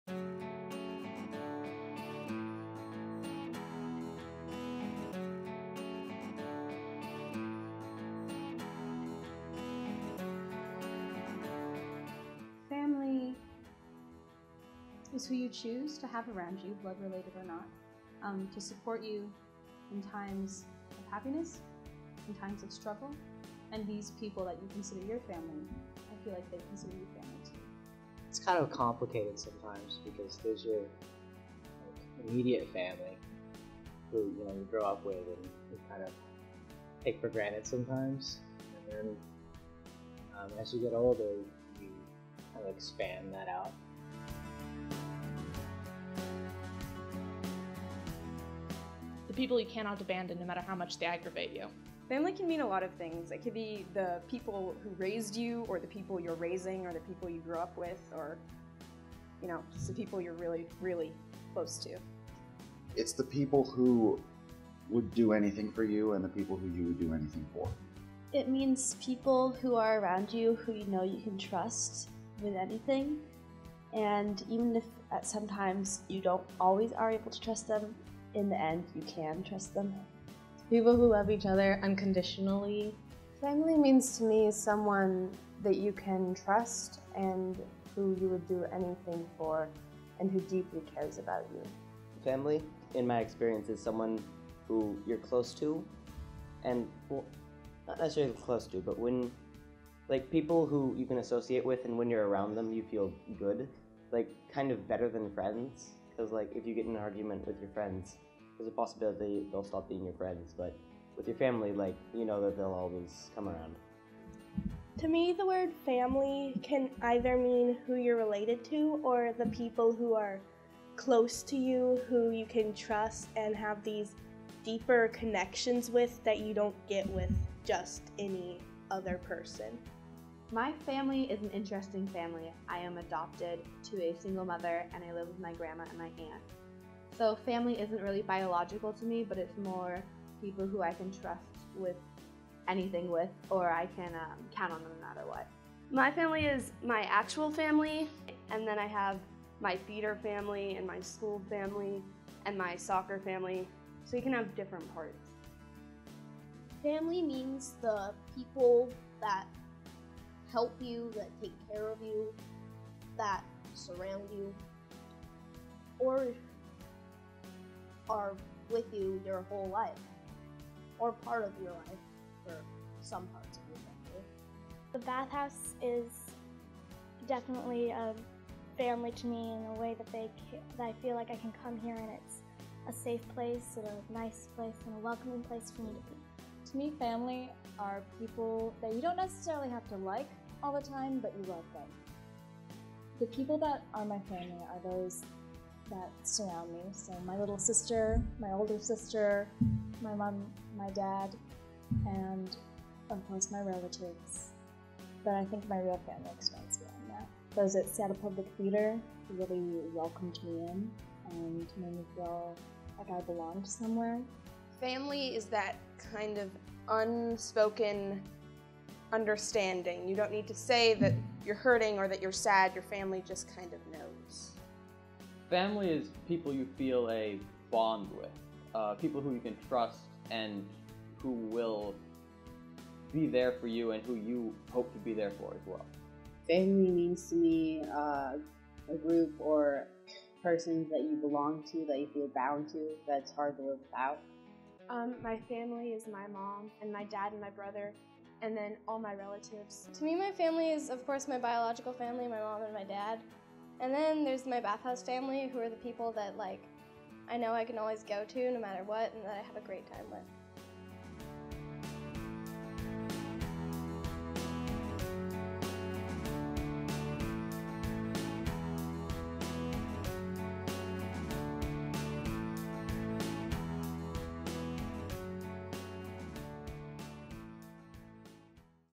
EdithVideo_noAuction_V1_youtube720
Directed and edited by David Gassner the cast of SPT's EDITH CAN SHOOT THINGS AND HIT THEM (also directed by David Gassner) and members of the SPT Youth Ensemble share their thoughts about the themes and insight of this amazing play.